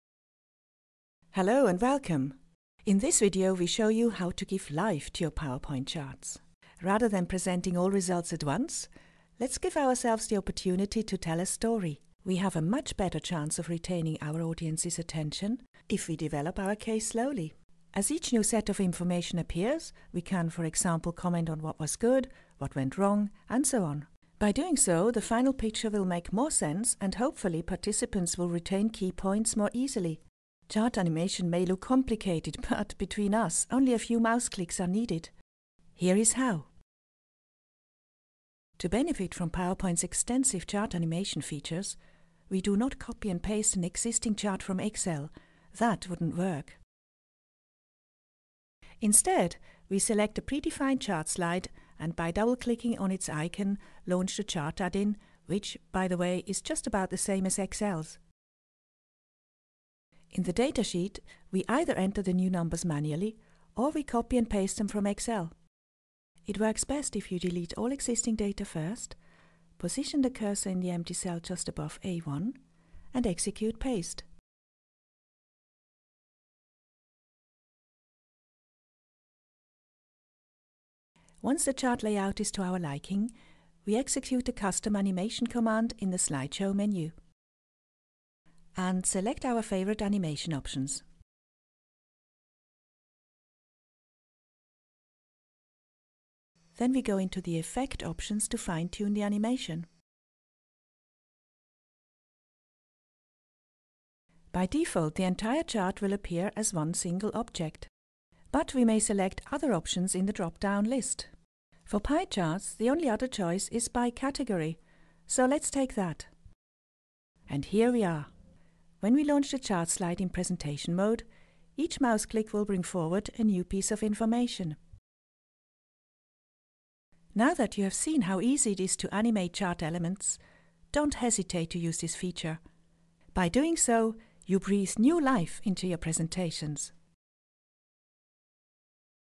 Animate your charts in Powerpoint
How to give life to your presentations by taking advantage of the Powerpoint animations features